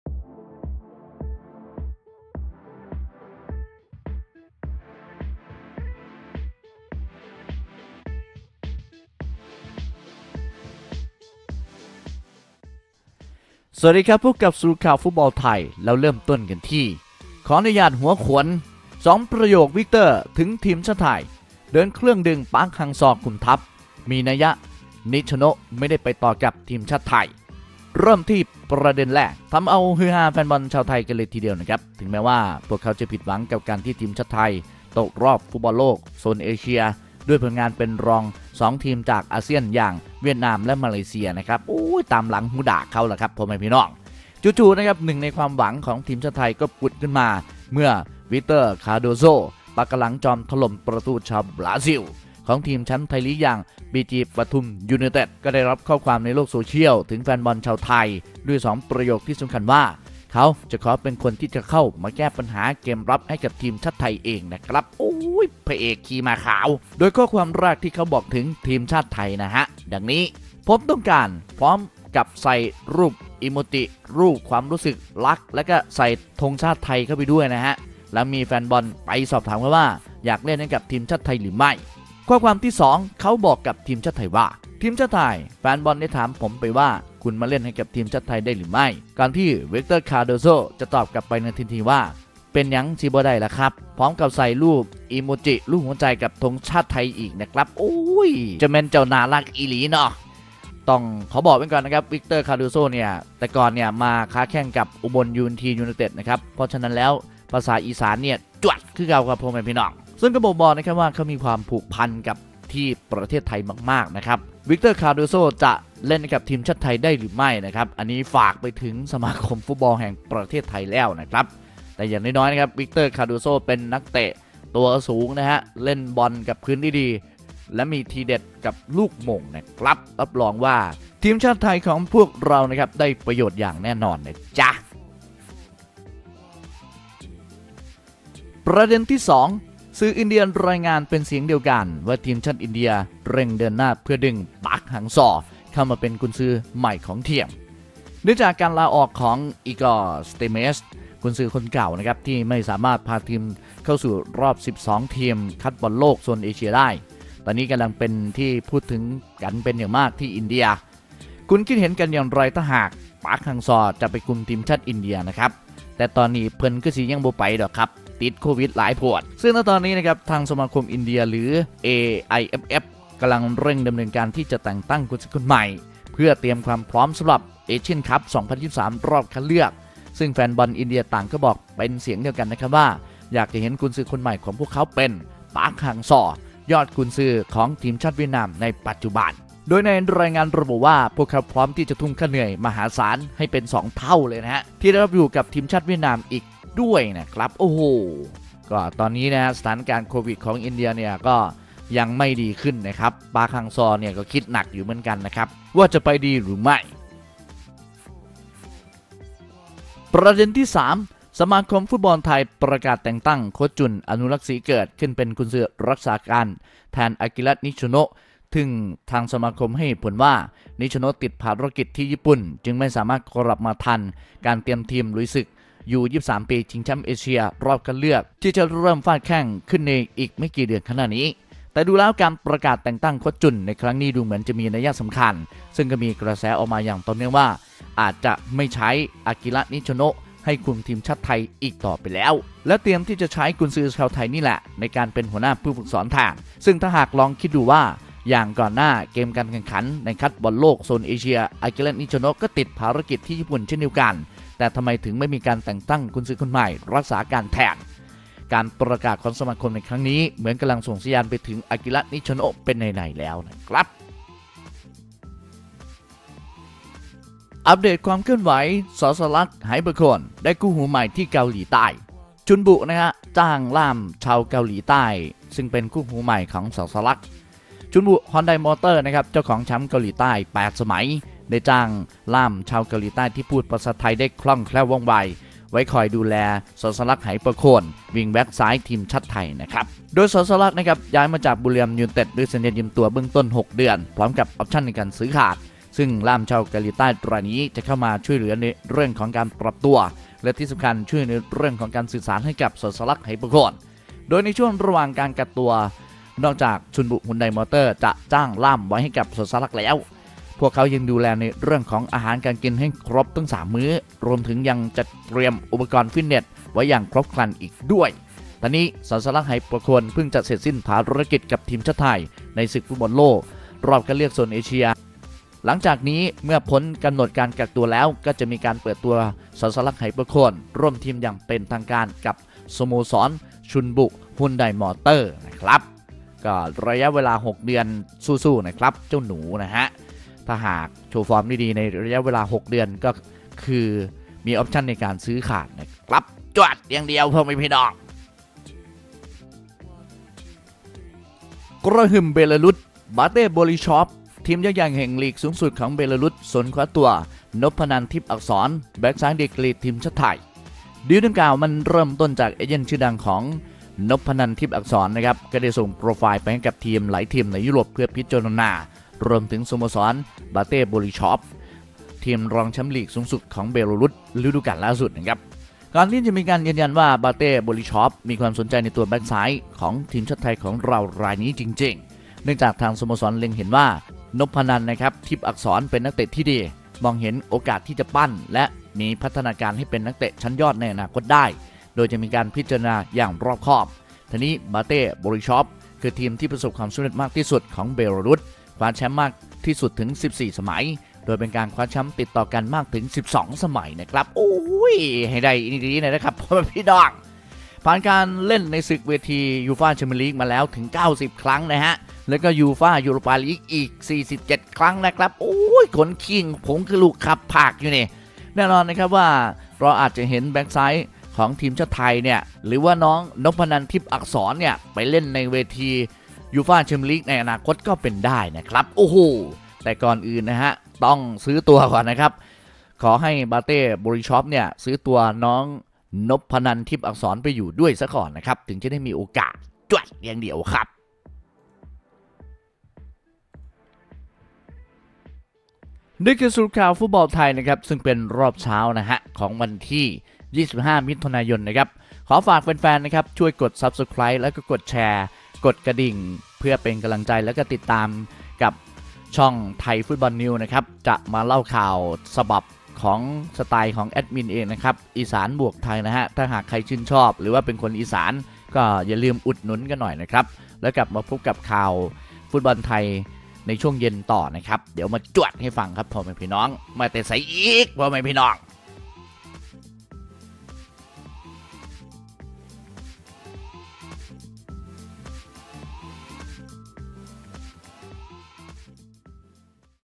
บาเต้ โบริซอฟ สนแบ็คซ้ายไทย/2 ประโยค "วิคเตอร์" ถึงทีมชาติไทย/เดินเครื่องดึง ฮัง ซอ คุมทัพ
เฟสบุ๊ค         ► Thai Football News
ยูทูป            ► Thai Football News